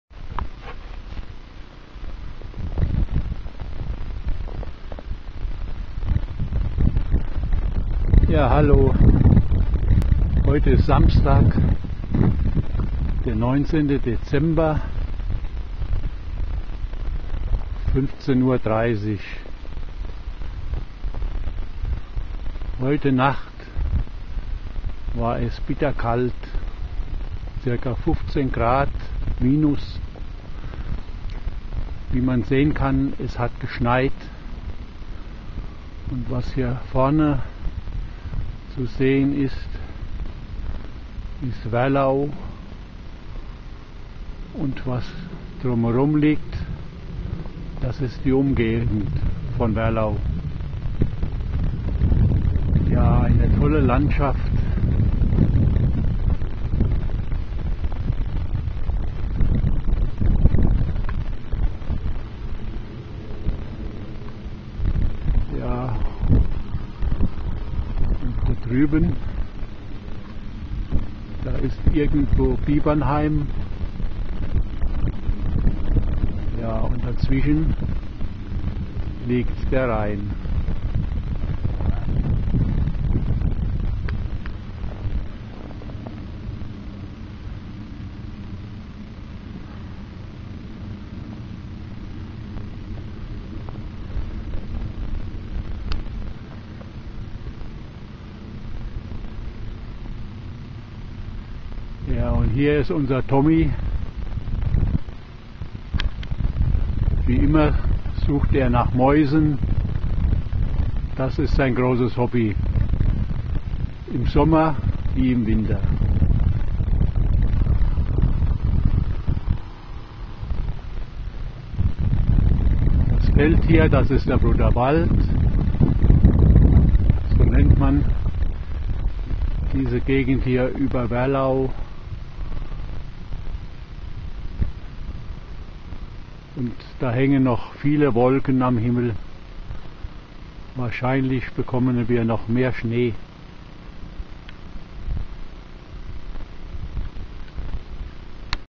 Werlau im Winter am19.12.2009.MOV
Ein Hundespazirgang im Winter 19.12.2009 Bruderwald in Werlau bei St.Goar am Rhein